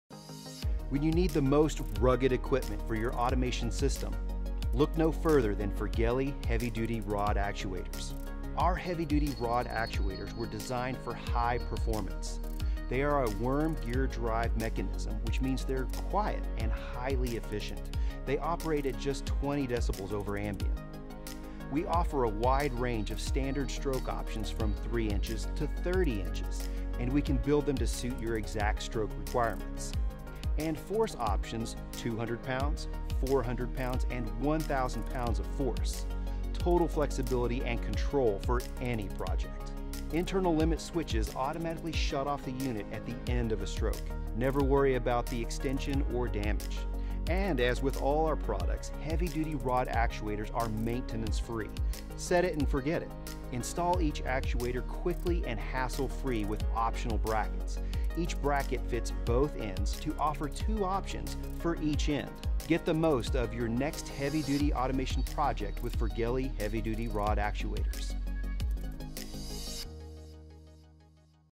Heavy Duty Actuators 101 - Everything you need to know about Firgelli Heavy Duty Actuators
Educate yourself before you buy a heavy duty Linear Actuator. These Actuators offer force options of 200, 400 and 1000lbs forces. We share with you the benefits of these 12v dc Linear actuators, so that you can make better buying decisions the next time your ready to buy one.